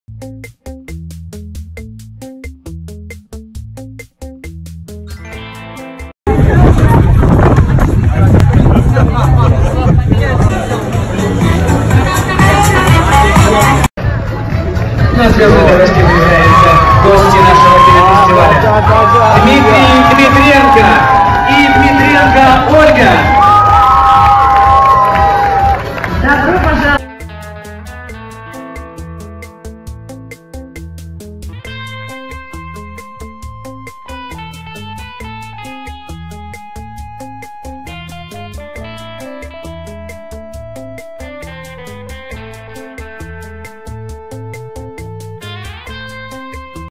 Рапунцель и Диму встречают овациями во Владивостоке (ondom2.com)
В этом выпуске новости и слухи  про  Ольгу Рапунцель и Дмитрия Дмитренко, которых встретили овациями во Владивостоке.